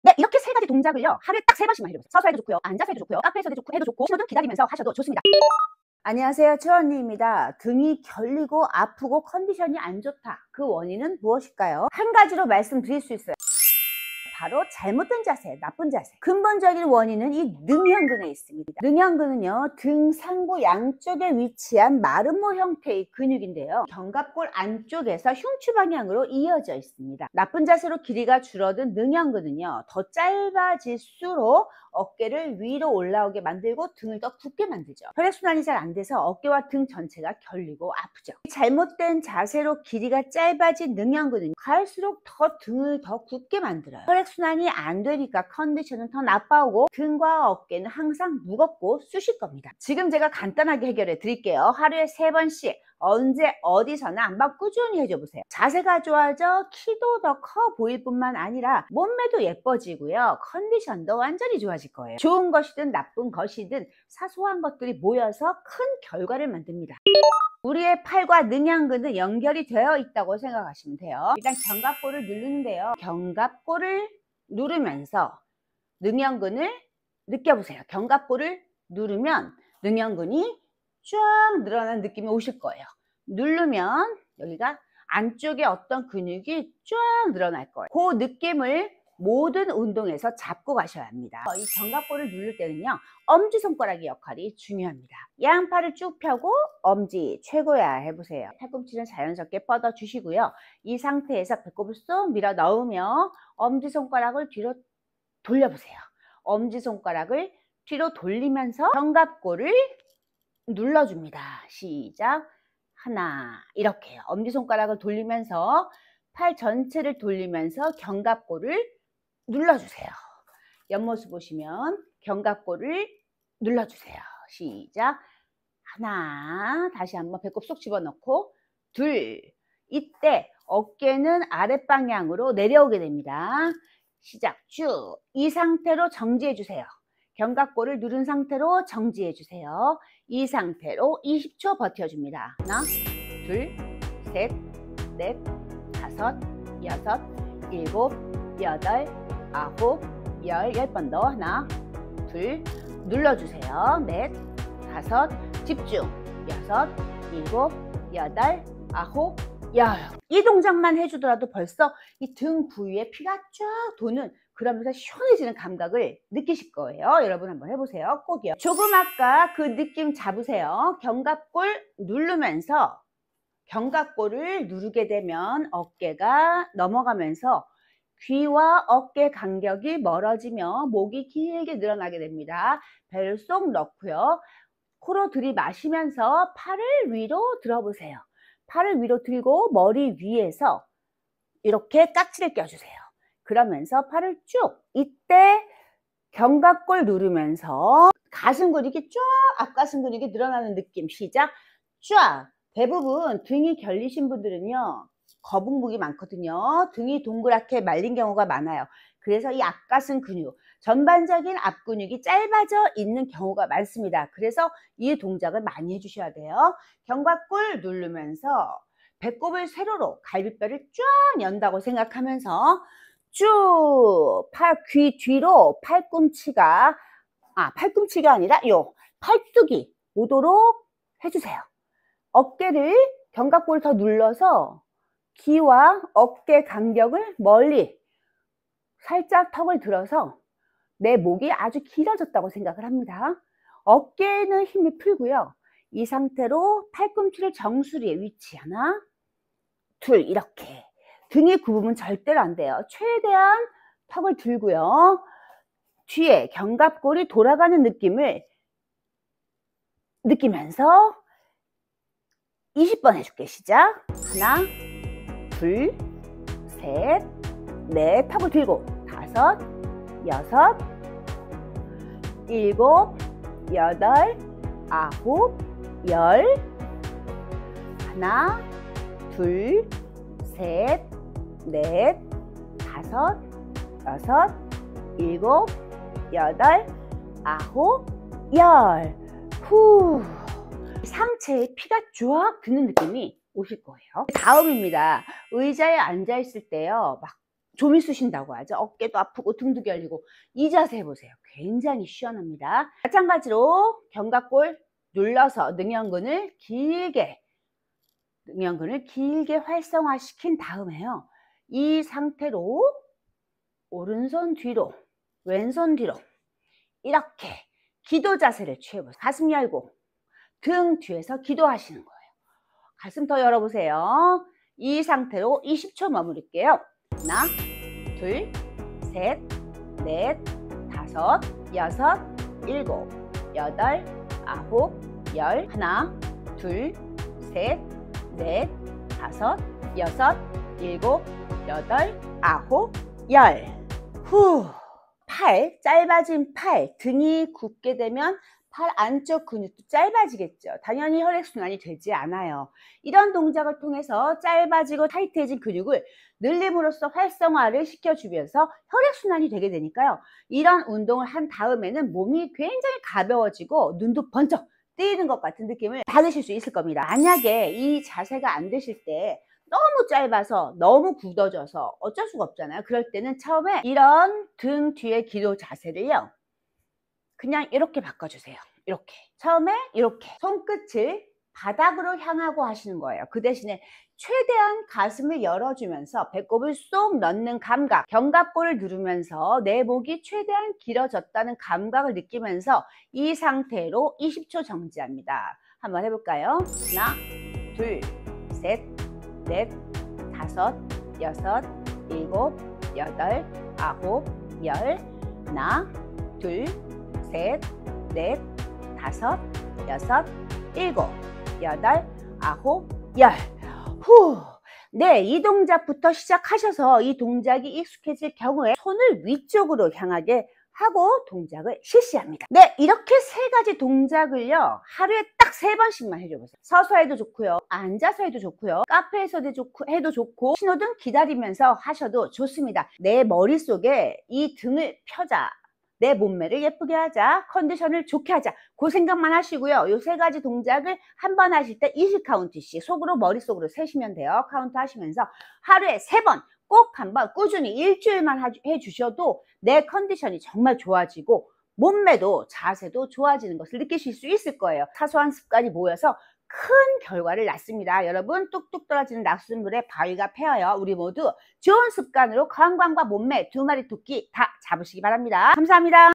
컨디션이 최악일때 20초만해보세요
등이 결리고 아픈이유를
한가지로 말씀드릴 수 있어요
 자세가 잘못돼서!
근본적인 원인은 능형근
능형근은 등상부양쪽에 위치한 마름모형내의 근육
견갑골 안똑에서 흉추방향으로 이어져 있습니다
길이가 줄어든 능형근은
어깨를 위로 잡아당기며
등을 더 굽게 만듭니다
갈수록
혈액순환이 안되니
항상 무겁고 뻐근하게 만들며
갈수록 등은 굽고 어깨는 올라오며 키까지 줍니다
간단하게 해결해드릴겁니다
하루에 3번씩
어디에서나 해주어보세요
자세가 좋아져
몸메도 도 이뻐보일 뿐 아니라
키고도 더커지고
컨디션도 좋어질 겁니다
좋은것이든
나쁜것이든
사소한 습관들이 모여 큰 결과를 만드는 법이거든요!채널 회원이 되시면
거꾸로 가는 시계 추언니 펠트니스 운동을
더욱 체계적이고 자세하게 배우실 수 있습니다^^
(가입하여 혜택을 누려보세요!)

# 등어깨쑤실때#거북목